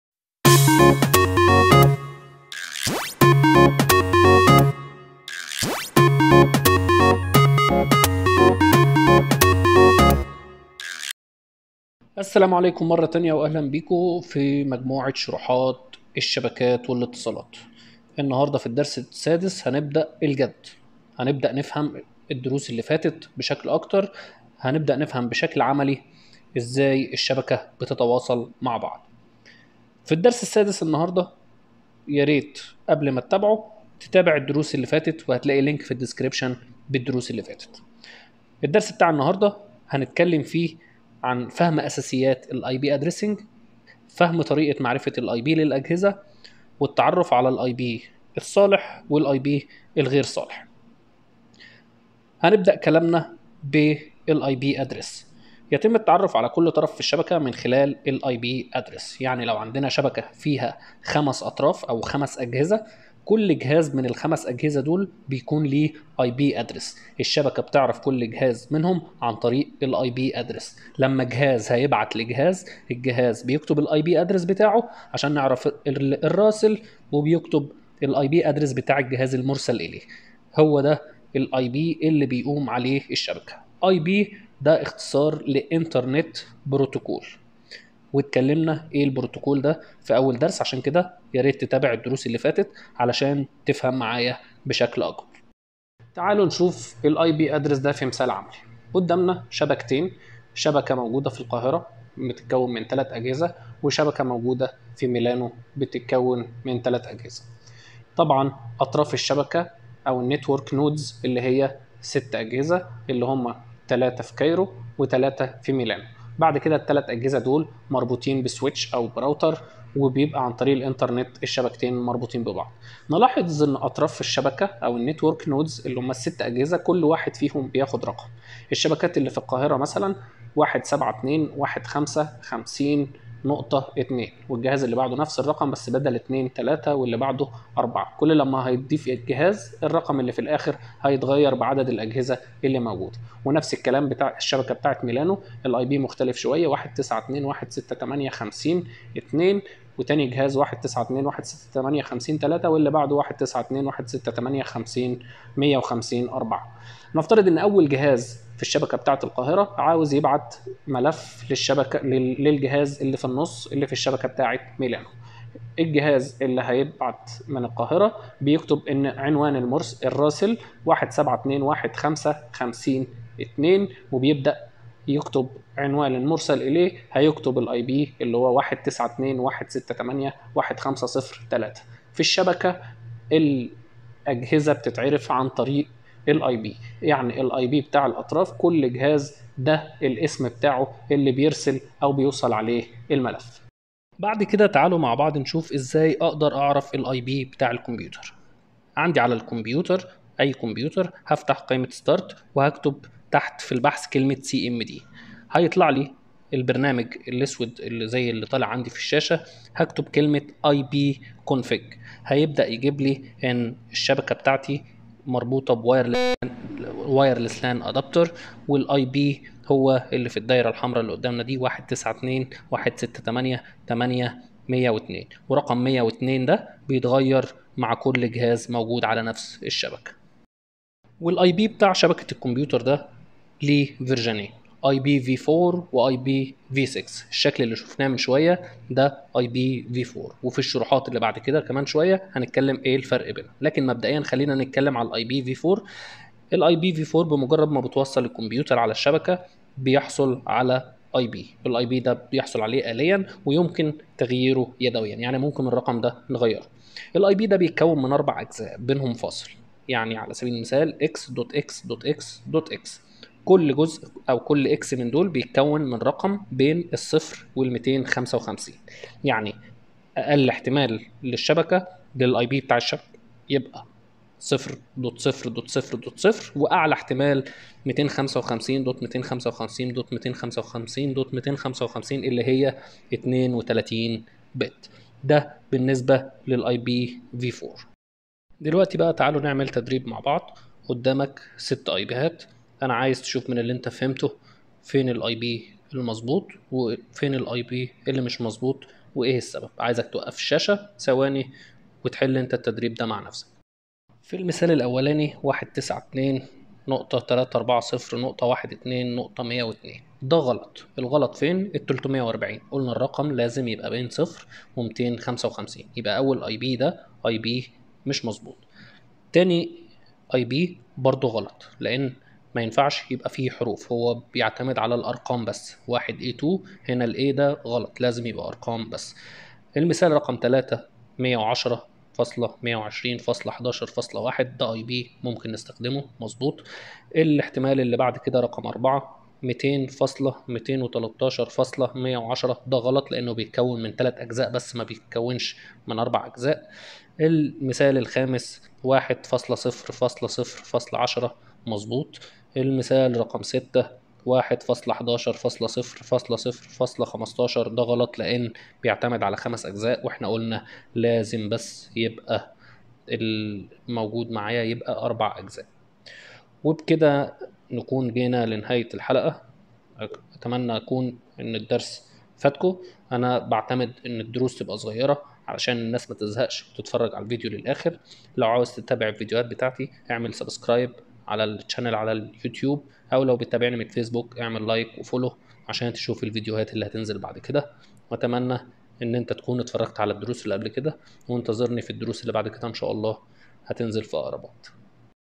شرح كامل للشبكات والاتصالات - الدرس السادس IP addressing
يعني ايه IP
ماهو IPv4
كيف يعمل الاي بي
شرح كامل للشبكات والاتصالات